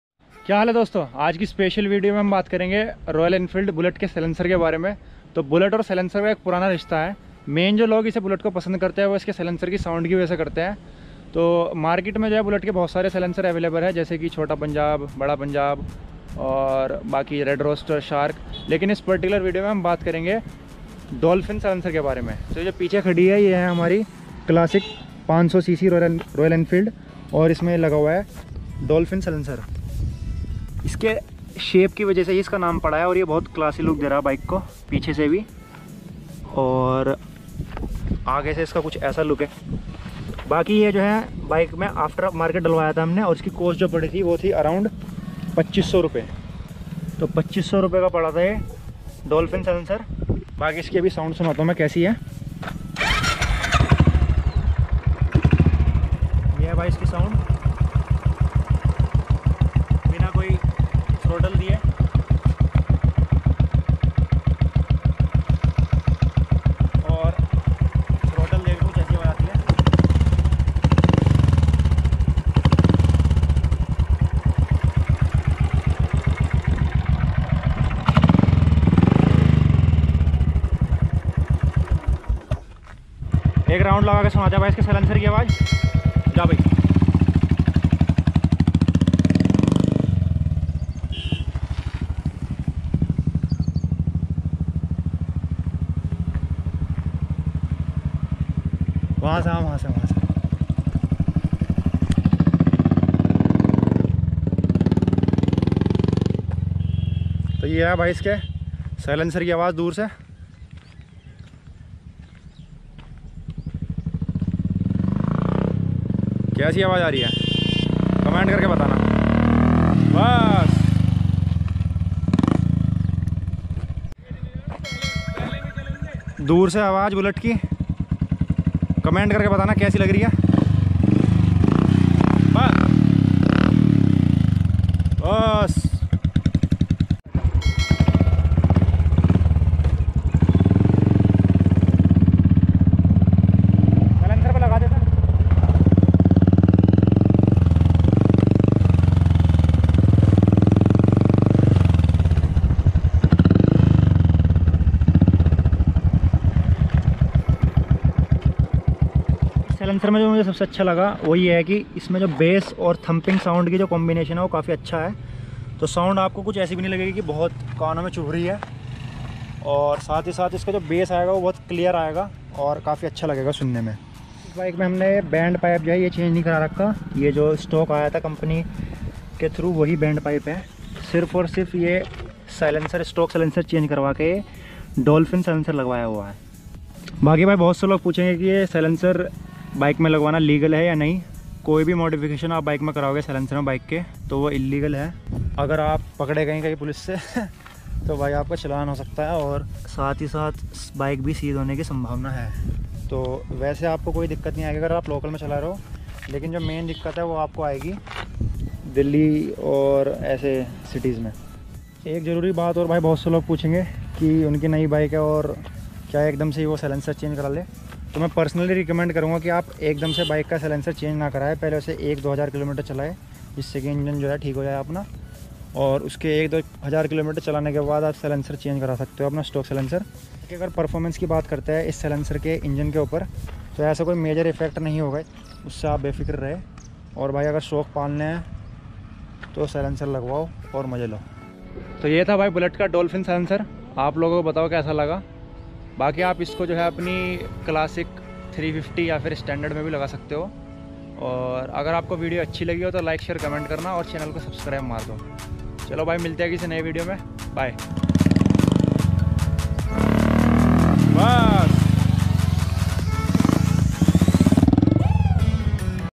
Dolphin Silencer on Bullet | Sound Test, Cost & Performance 🔥
In this video, I have reviewed dolphin bullet silencer. Feel the heavy bass and dug dug sound 😉😎 If you are planning to use the same silencer on your Royal Enfield motorcycle, then watch out this video.

Video kaisi lagi, comment krke btane 💬
Like ar Share bhi mar diyo bhaiyo ✌🏻